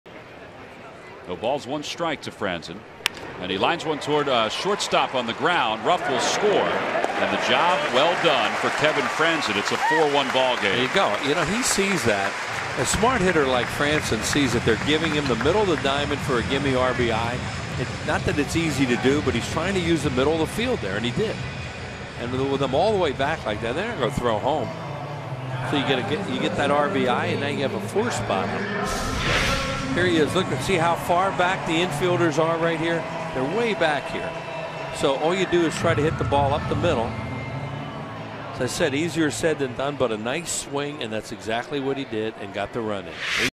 ARI@PHI: Frandsen pads early lead with RBI groundout
8/25/13: Kevin Frandsen grounds out to short to allow Darin Ruf to score, giving the Phillies a 4-1 lead in the first